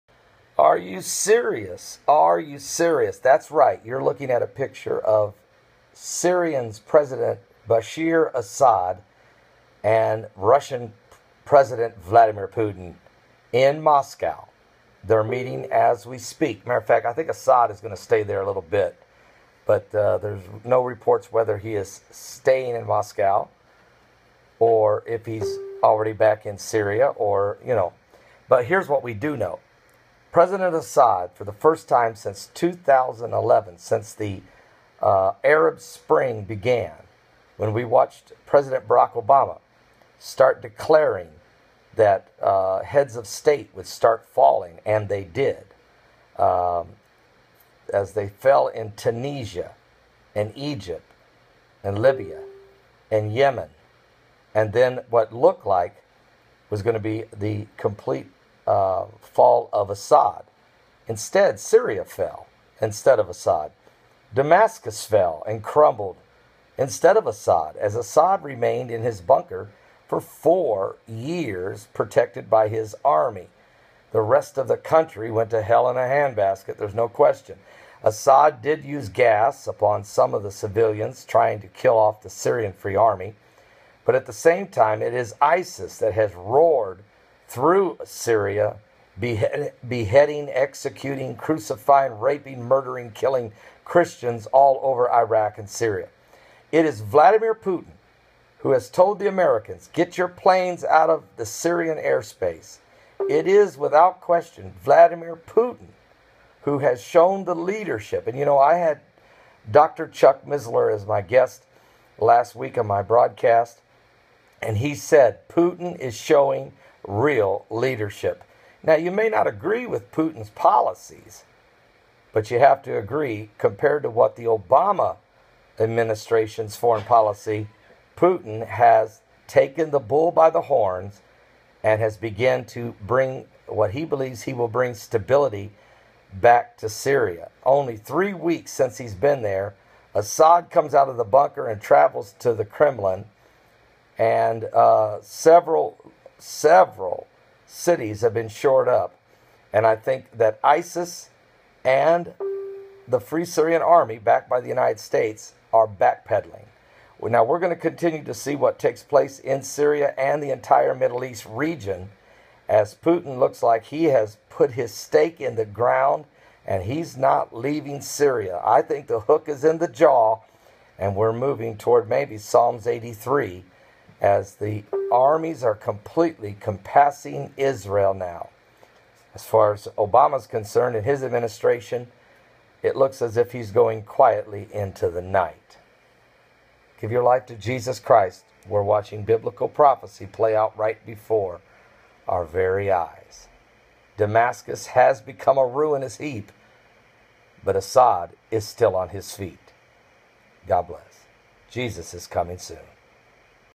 Syria's Assad Goes To Moscow
Syrian President Assad flies to Moscow to meet with Russian President Putin also